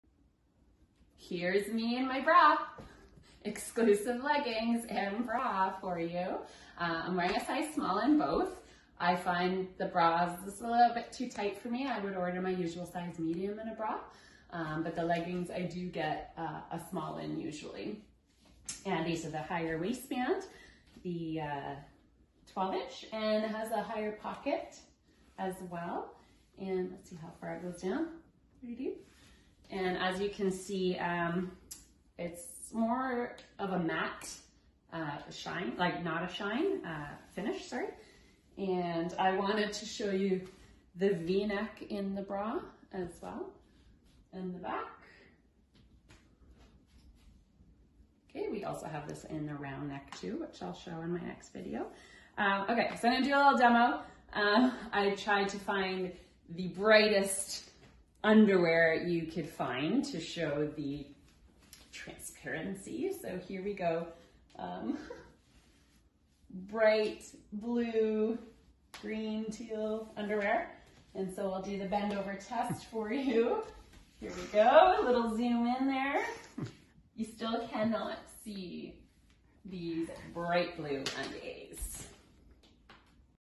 8-Way Matte Exclusives Stylist Review
Discover 8-Way Matte! This multi-functional, moisture-wicking fabric is ideal for working out as well as everyday wear. With a sleek matte finish, these leggings feature medium compression and won’t slip and slide. Available in both 9.5” and 12” waistbands, this brand-new fabric also provides great coverage.